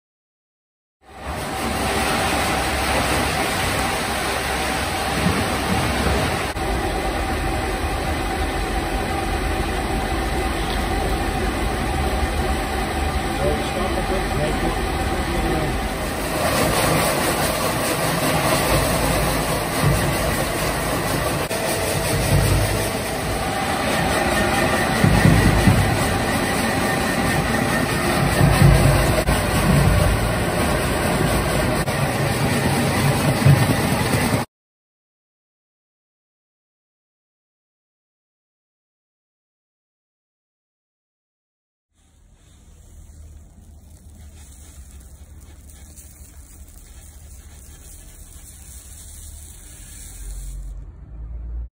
Cereal deboxing and depackaging
Out of date and out of spec cereals being de-packaged and used in animal feed.
The feed was over 99.85% pure